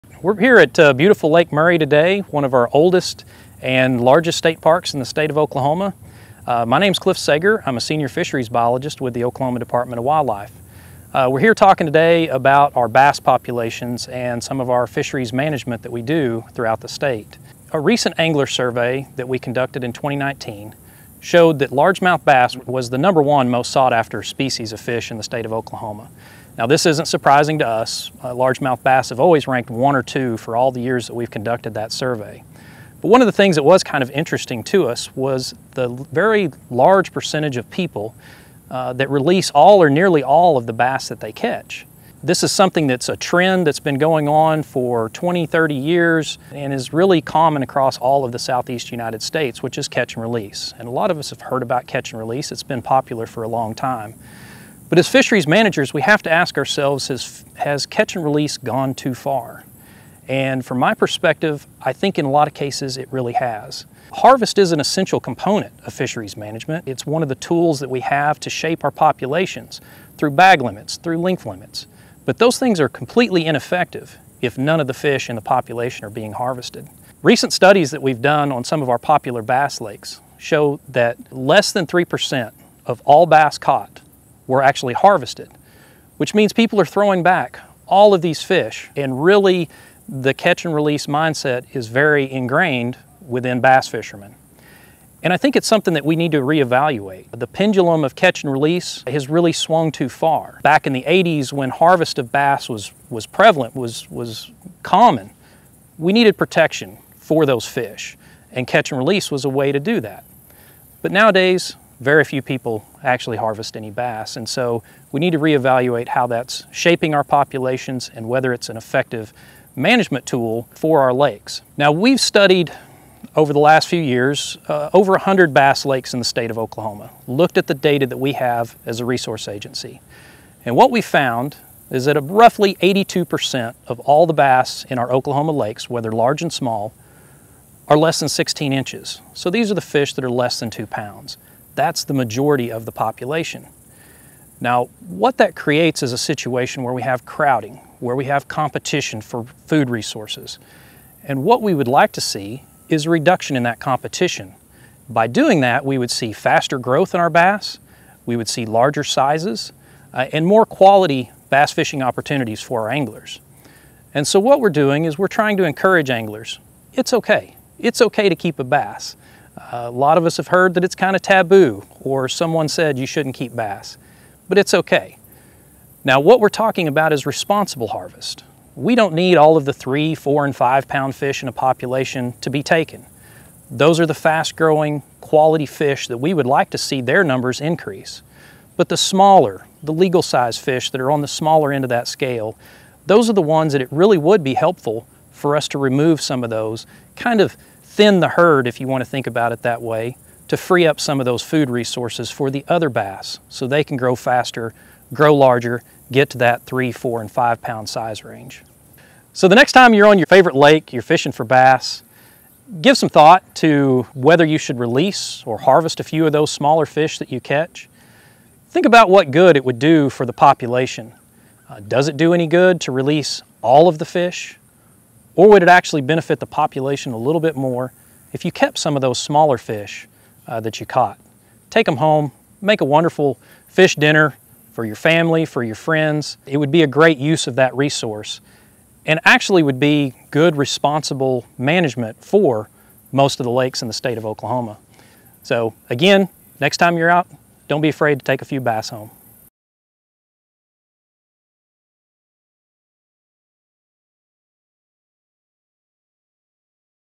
It's okay, keep a bass!
In the most recent survey of OK anglers, black bass ranked as the #1 most sought-after species.  But something else in that survey was troubling to fisheries biologists; nearly 70% of anglers release ALL the bass they catch. Now why is this important? Without harvest, fisheries regulations aren’t very effective at shaping and improving the quality of fish populations. Responsible harvest is often a necessary part of the management puzzle.